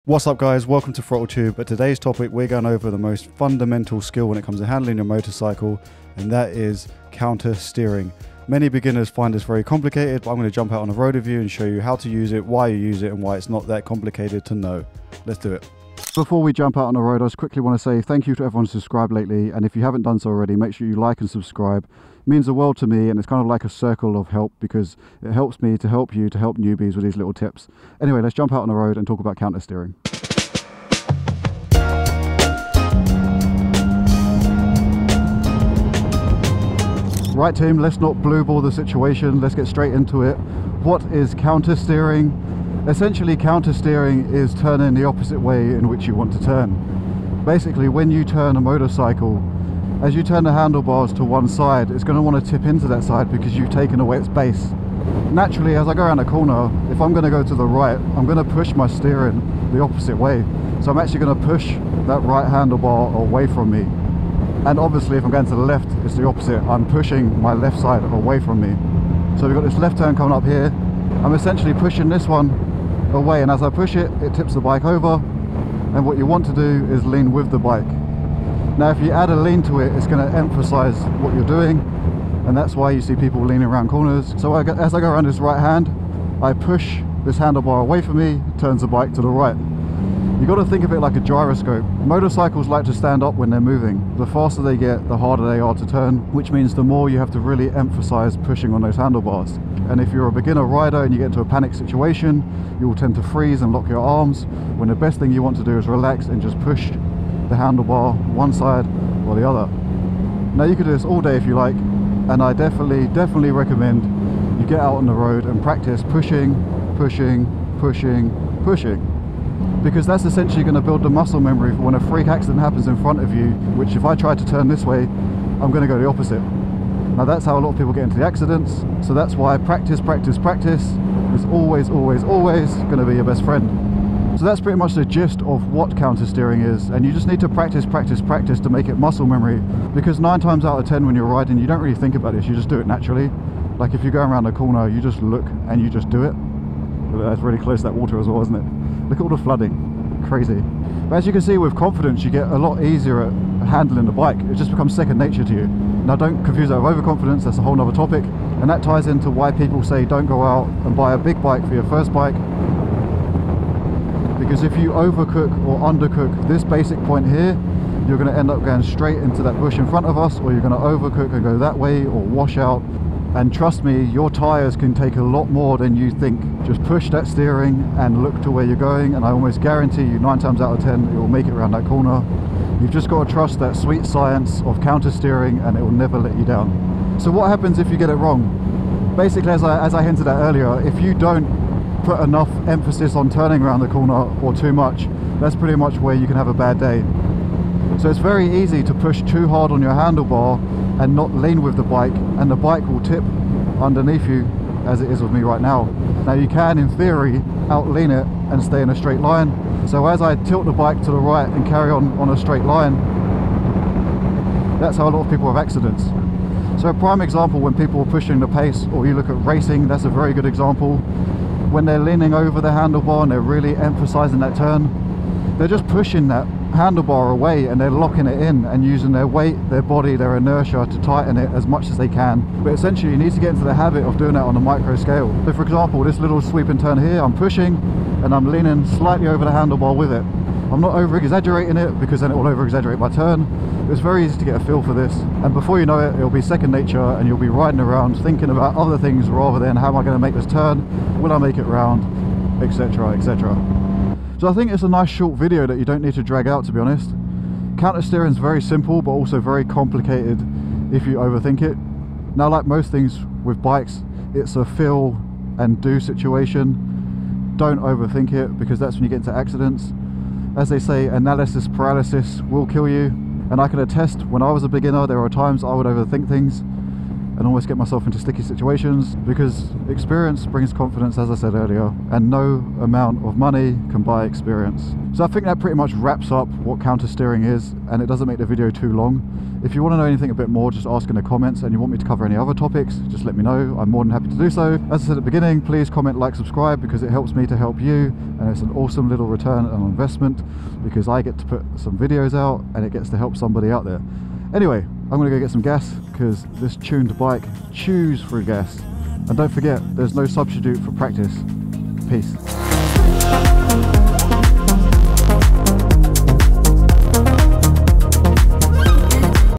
Corner With Confidence | Countersteering Tutorial
Countersteering is arguably one of, if not the most fundamental handling skill every rider must master with their motorcycle. From do’s and don'ts to gaining confidence in twisting turns, this video has you covered. So what are you waiting for? Get out there and practice holding some sweet riding lines. And remember, don’t forget to lean with the bike and trust your tyres!

*Everything 2 Wheels*

► *CHAPTERS*
00:00 - Intro
00:32 - What Is Countersteering?
01:00 - The Correct Way
02:30 - Some Great Advice
03:06 - Do NOT do this!
04:14 - Summary
04:48 - SUBSCRIBE/Request Video Topics